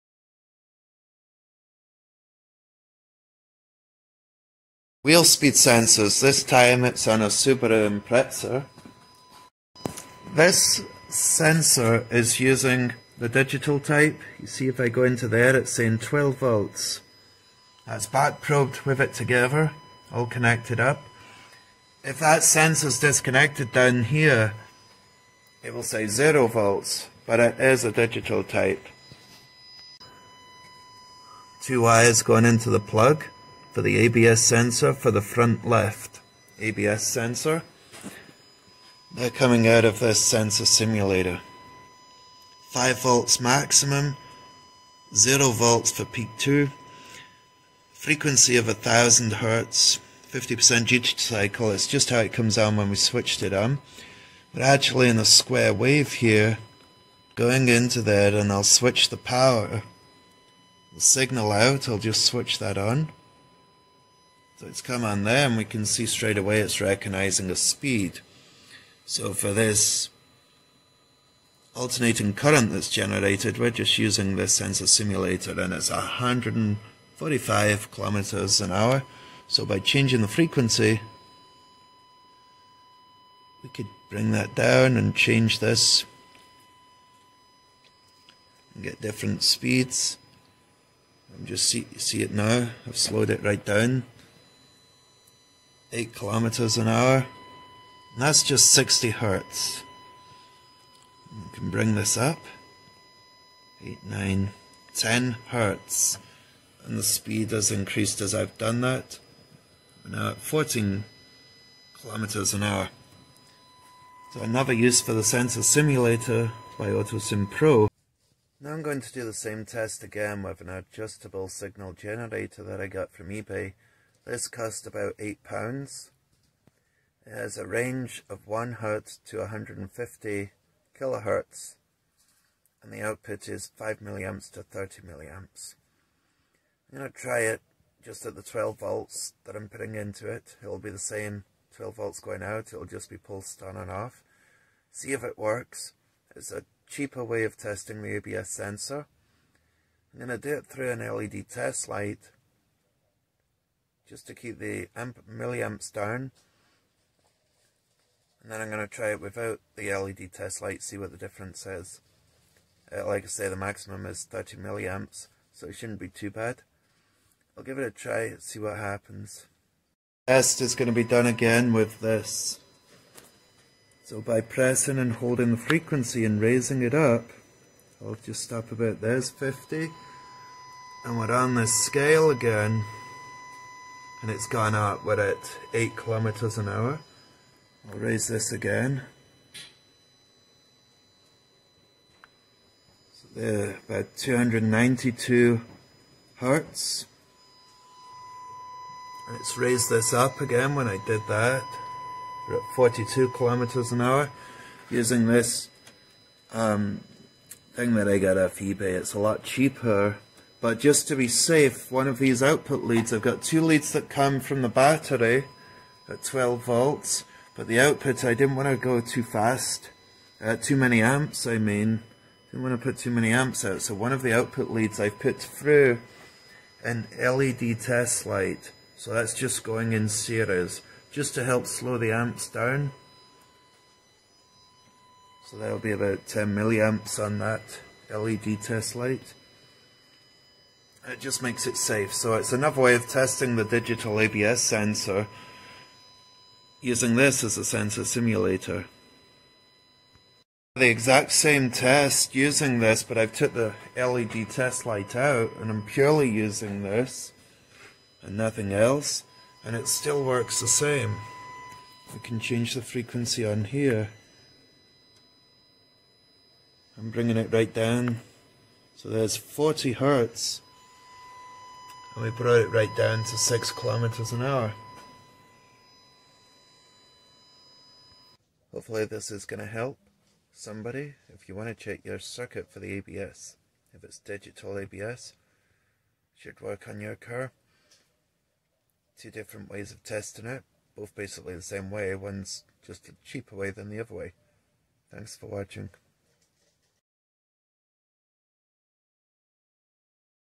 ABS Sensor Simulator (digital ABS) doesn't work on all cars
hi I'm Matt, I am checking an active abs sensor on a Subaru here. this method with the sensor simulator did not work when used on a Citroen/peugeot.
in this video I show two different ways to test the circuit of an ABS speed sensor, one with a sensor simulator, the AutoSim Pro from Ditex.  the other way was using an inexpensive adjustable signal generator that I bought from eBay.
both ways work fine for getting a speed signal on the scan tool.